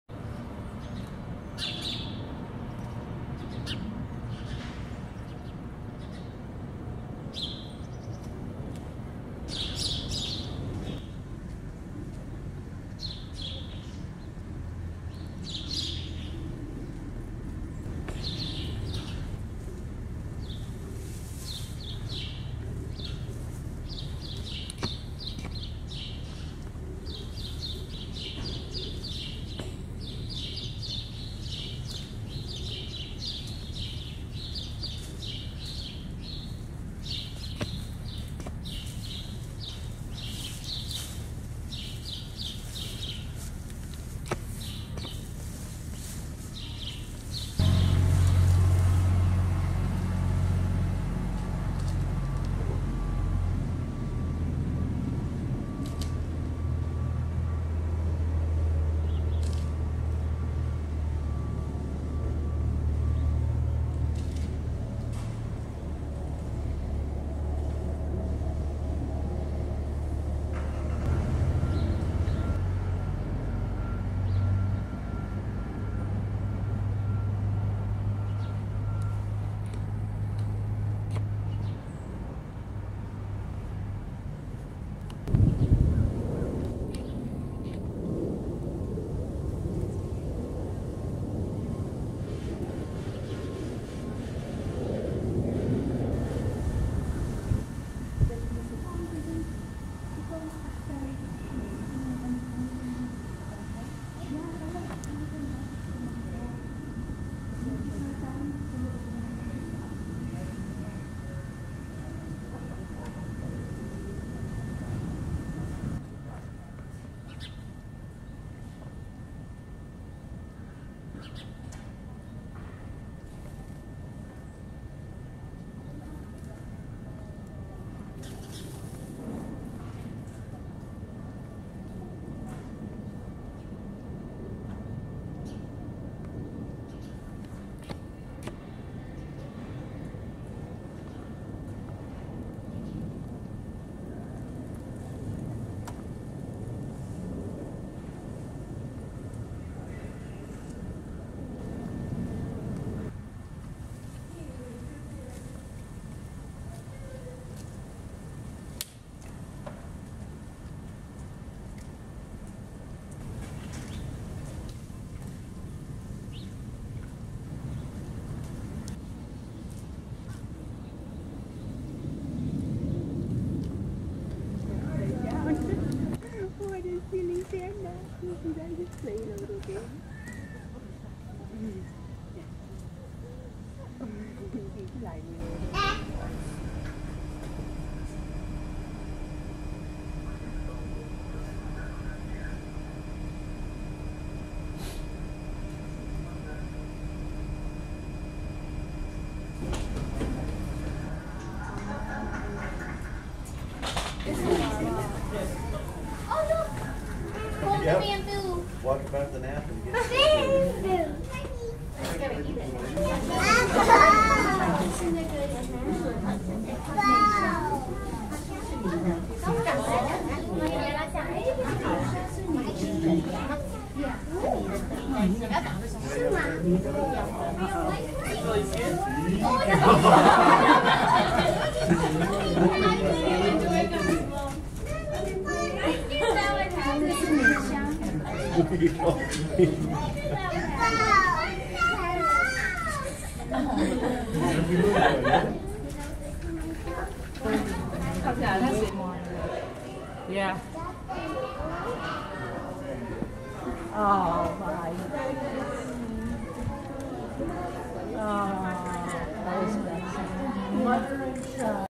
Bei Bei 1/25/17
Bei Bei and his mother, Mei Xiang. Recorded at the Smithsonian National Zoo on January 25, 2017.